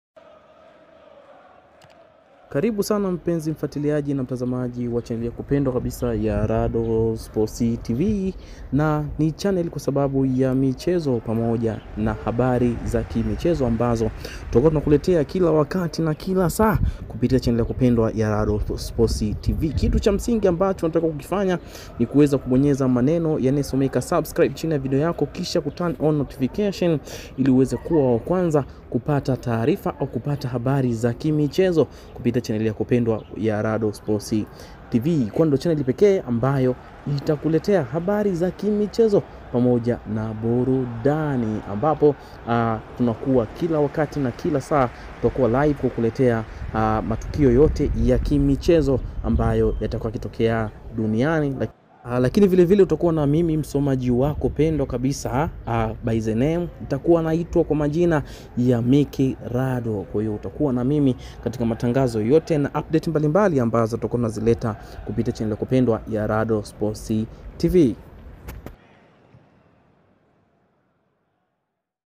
Kazakhstan vs Denmark (0-2)European Championship
Hojlund Goals,Kazakhstan vs Denmark (0-2)Goals Results and extended Highlights European Championship